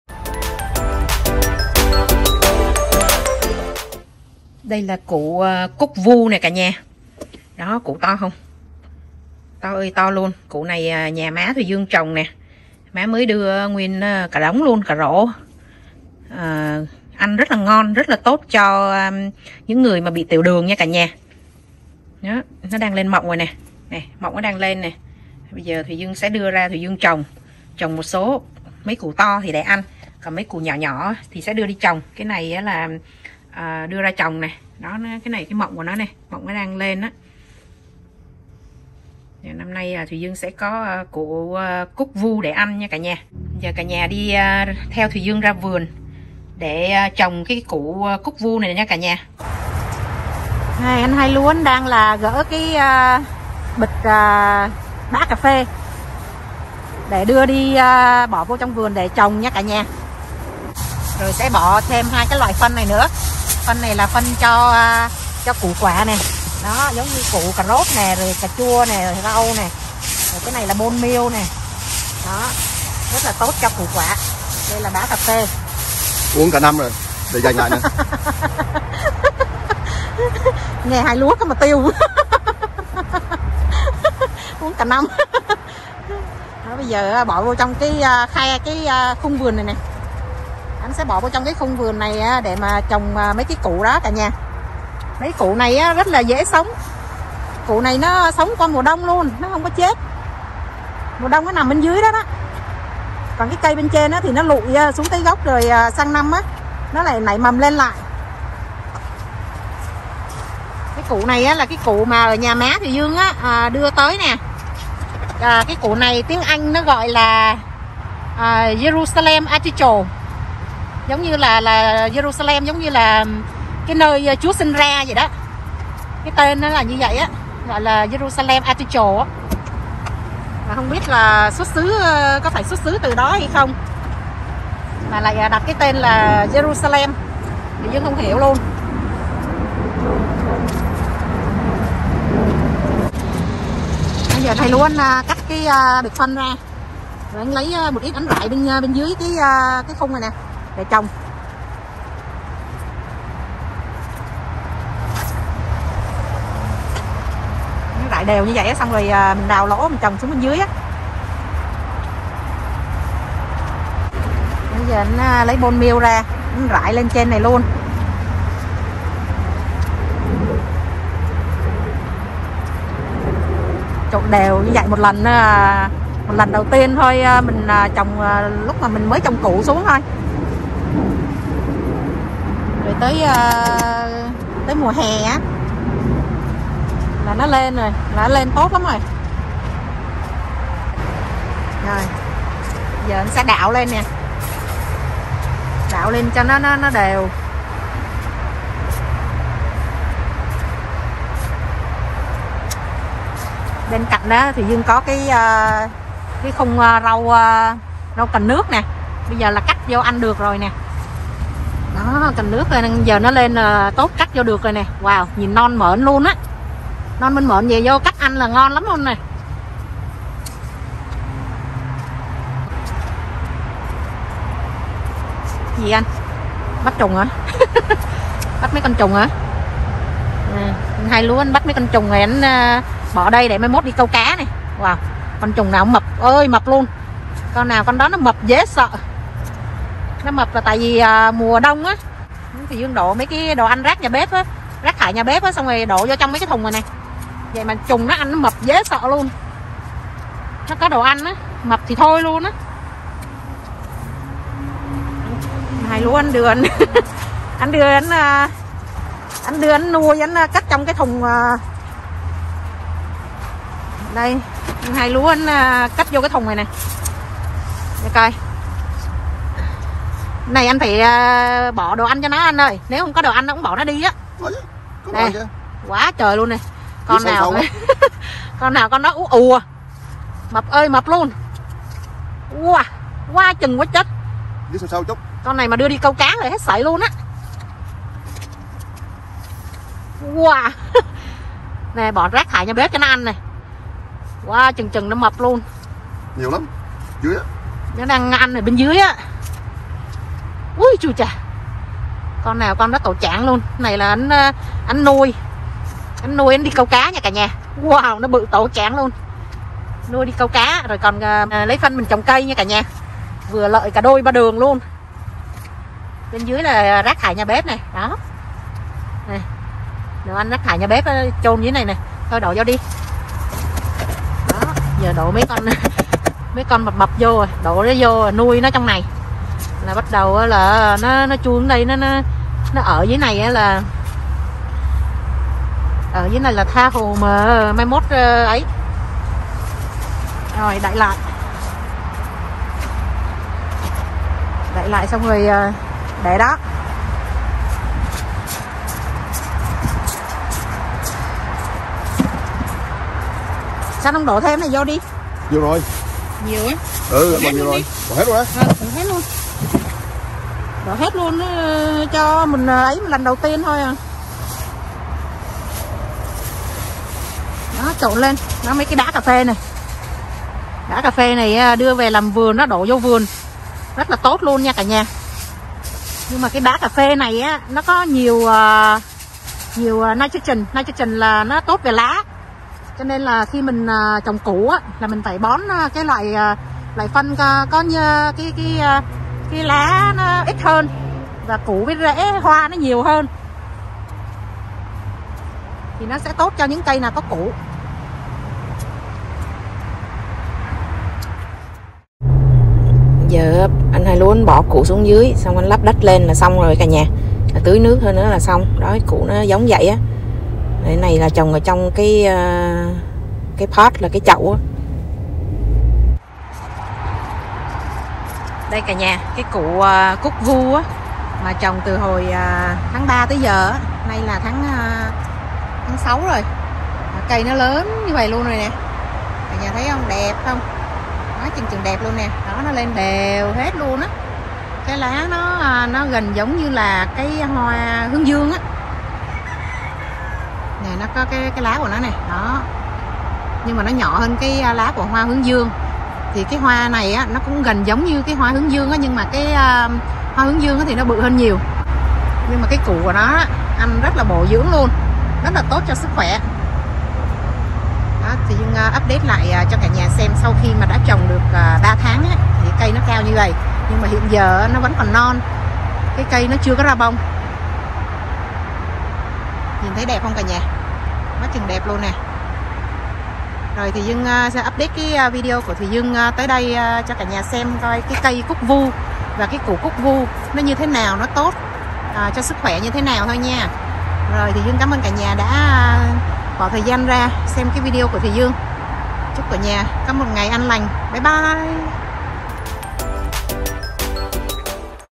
CÁCH TRỒNG CỦ CÚC VU & NUÔI TRÙN CÂU CÁ /JERUSALEM ARTICHOKE/ SUNCHOKE
củ cúc vu là một loại củ ăn rất tốt cho sức khỏe nhất là cho những người bị tiểu đường. Còn nuôi trùng cho mập là để đi câu cá và lấy phân bón cho cây trồng rất hiệu quả.